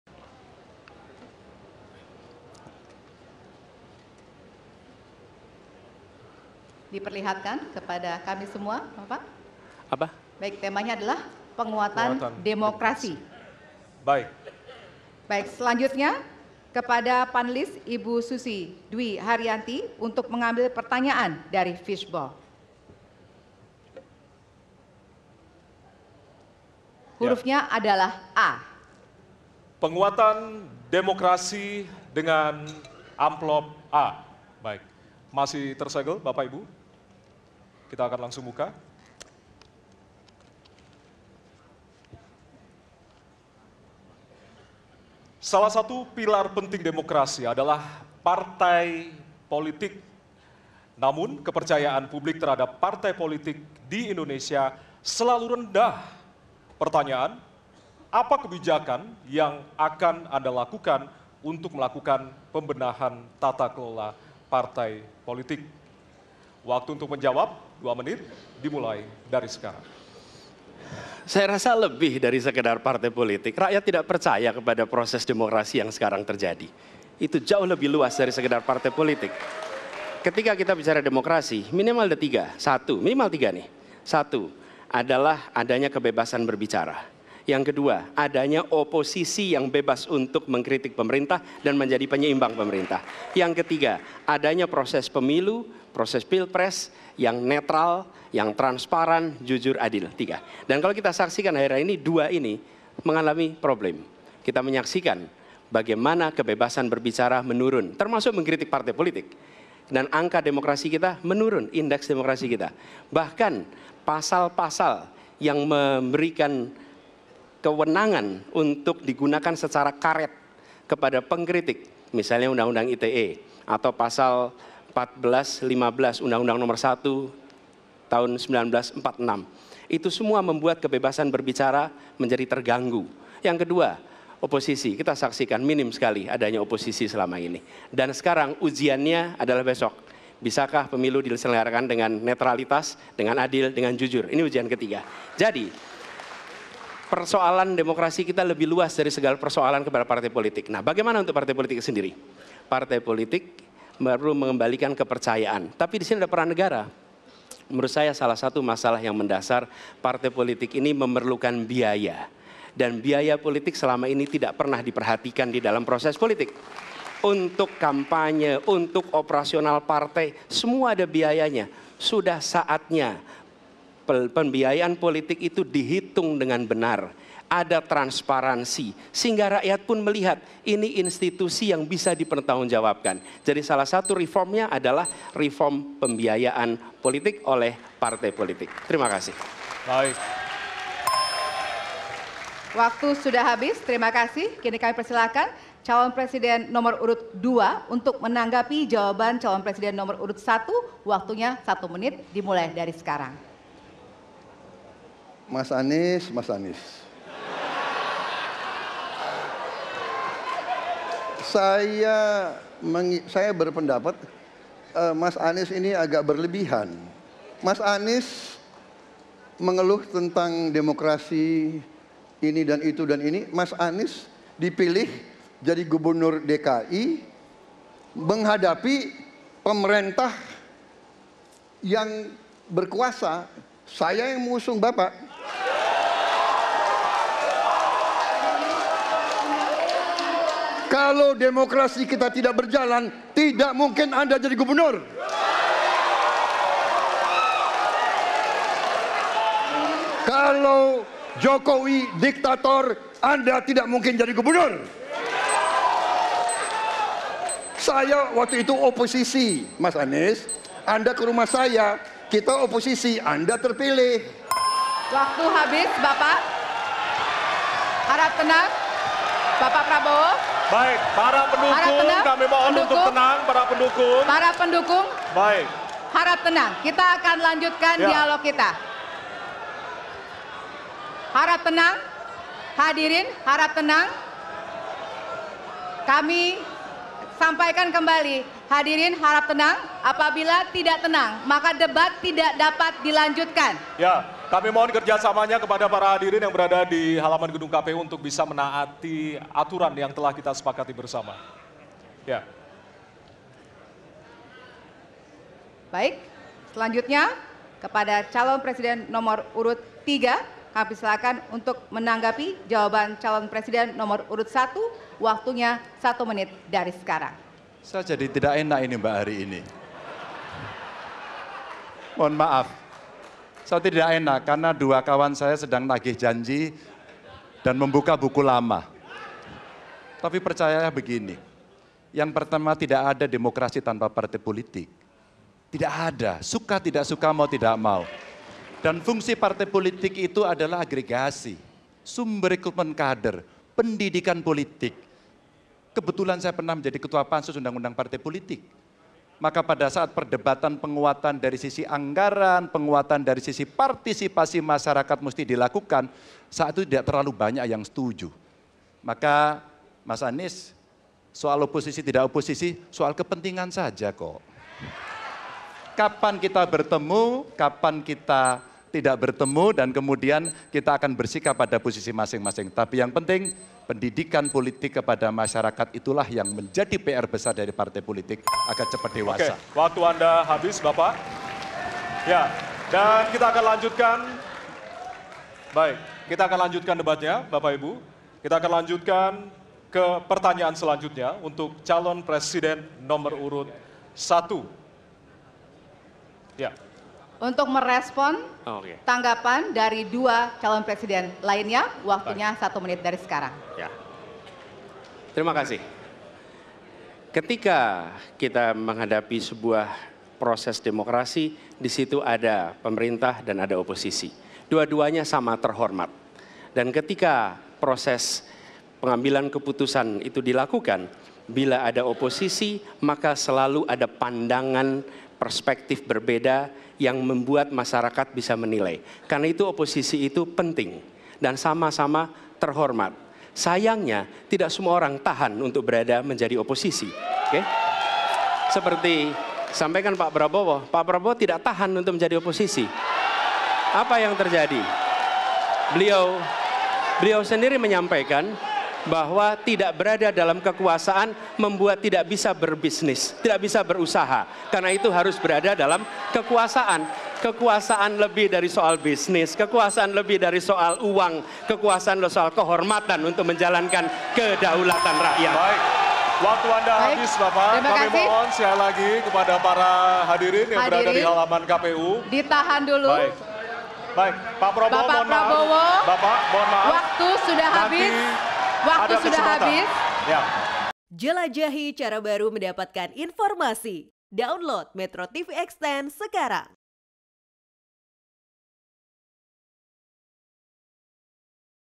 Isu Penguatan Demokrasi, Prabowo & Anies Buka Buku Lama
MetroTV, Komisi Pemilihan Umum telah menetapkan tanggal debat para paslon capres-cawapres 2024. Debat akan dilaksanakan menjadi lima kali, yakni tiga kali capres dan dua kali cawapres.

KPU juga telah menyiapkan mekanisme serta tema yang akan dibahas dalam debat tersebut. Di antaranya, Hukum, HAM, Pemerintahan, Pemberantasan Korupsi dan Penguat Demokrasi.